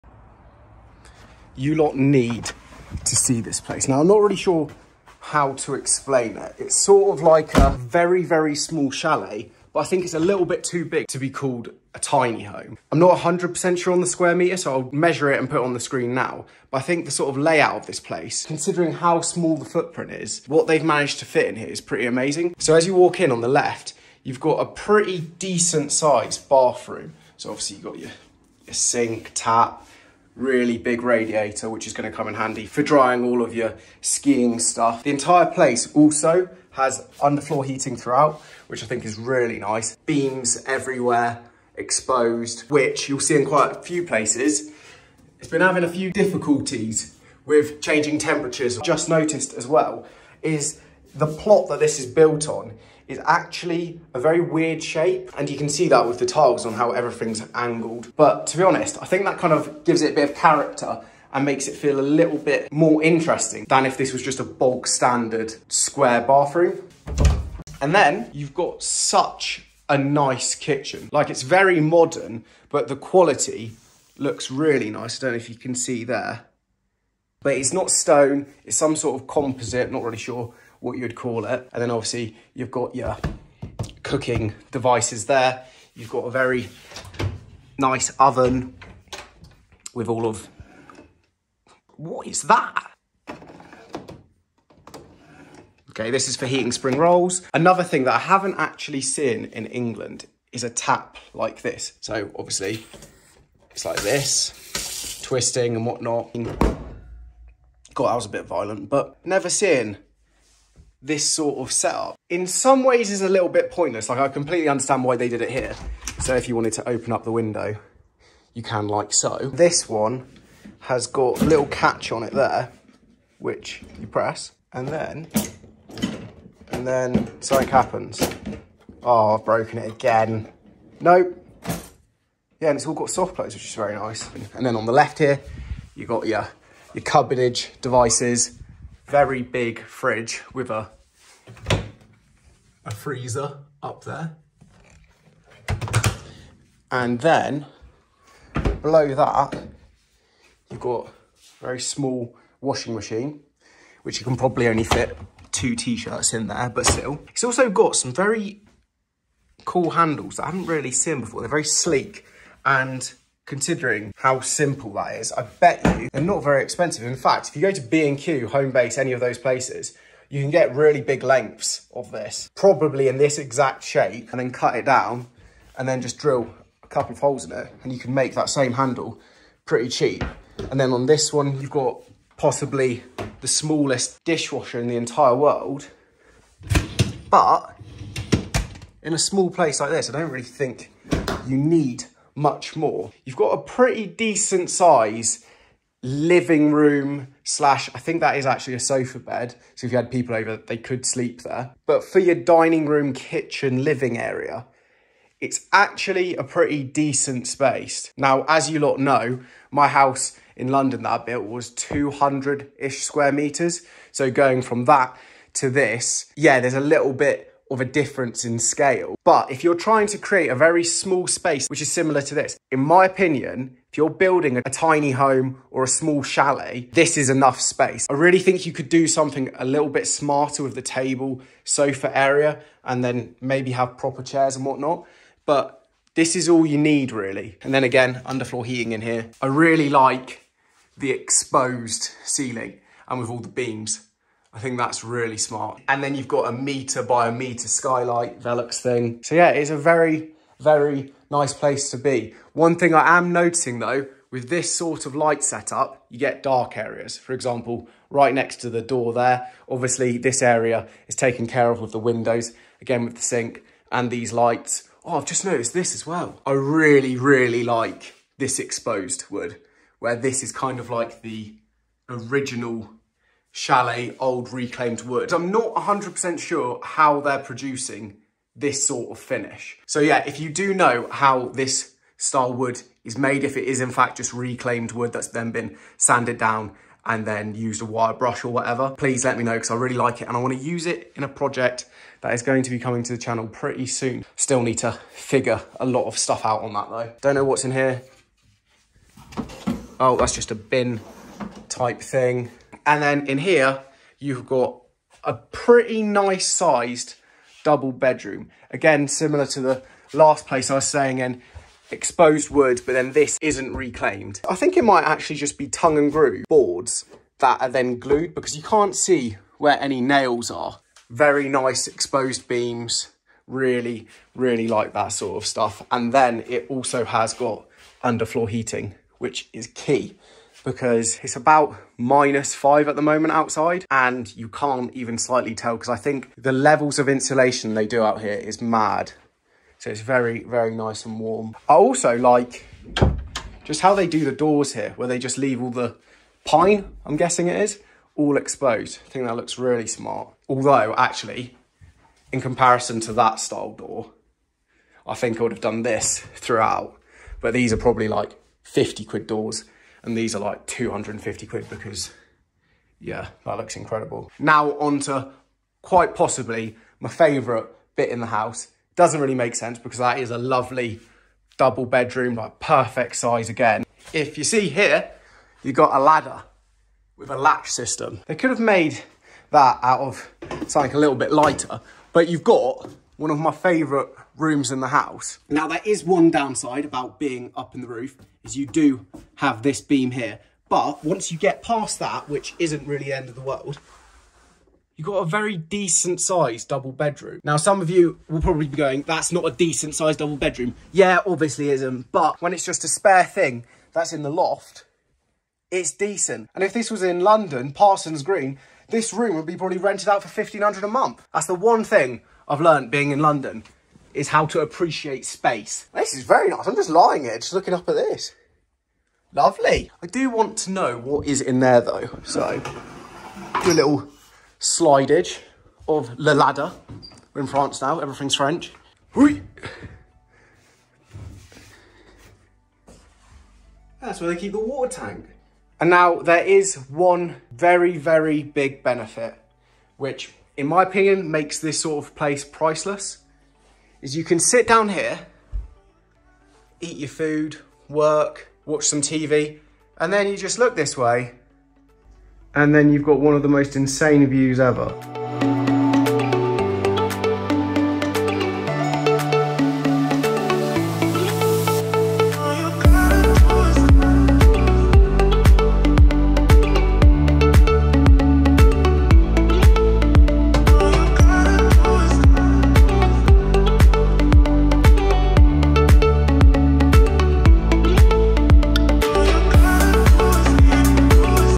Inside a Tiny House in the French Alps! - Home tour Chamonix
In todays video I had quick look around this small chalet/tiny house in the Alps.